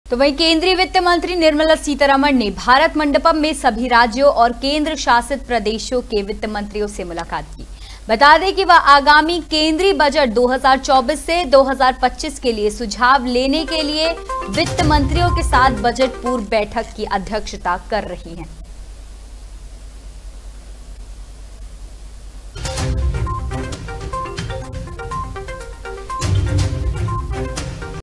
वित्त मंत्री ने सभी वित्त मंत्रियों से की मुलाकात
वित्त मंत्री निर्मला सीतारमण ने दिल्ली में वित्त मंत्रियों की मुलाकात की,
भारत मंडपम में सभी राज्यों और केंद्र शासित प्रदेशों से साझा किए विचार।
आगामी केंद्रीय बजट 2024-25 के लिए सुझाव लेने की तैयारी,
बजट-पूर्व बैठक में वित्त मंत्रियों के साथ आलोचना और विचार।

Finance Minister Nirmala Sitharaman convened a meeting of finance ministers in Delhi,
where discussions were held with finance ministers from all states and union territories, including legislative assemblies.
Preparations are underway for suggestions for the upcoming Central Budget 2024-25,
with deliberations and consultations with finance ministers in the pre-budget meetings.